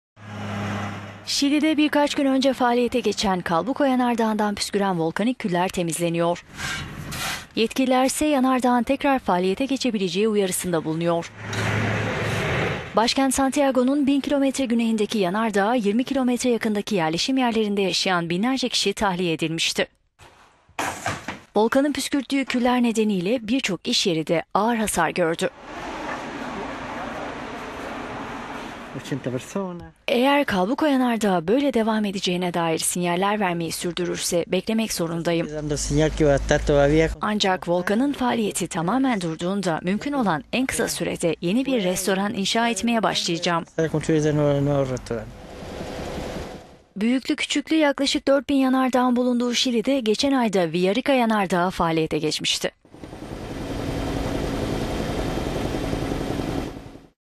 Şili'de Calbuco Yanardağı tekrar faaliyete geçebilir
Şili'de birkaç gün önce faaliyete geçen Calbuco Yanardağı'ndan püsküren volkanik küller temizleniyor. Yetkililer ise yanardağın tekrar faaliyete geçebileceği uyarısında bulunuyor.

Başkent Santiago'nun bin kilometre güneyindeki yanardağa 20 kilometre yakındaki yerleşim yerlerinde yaşayan binlerce kişi tahliye edilmişti.

Volkanın püskürttüğü küller nedeniyle birçok iş yeri de ağır hasar gördü: "Eğer Calbuco Yanardağı böyle devam edeceğine dair sinyaller vermeyi sürdürürse, beklemek zorundayım. A…